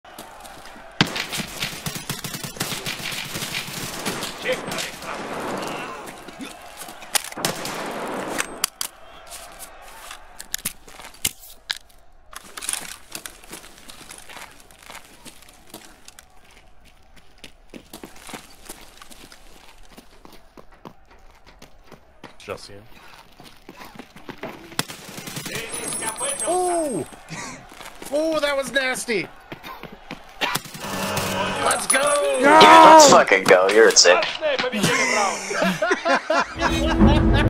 Clutching in Chop Shop
Clutching the round when you're barely last alive.

The SA-58 is GOATed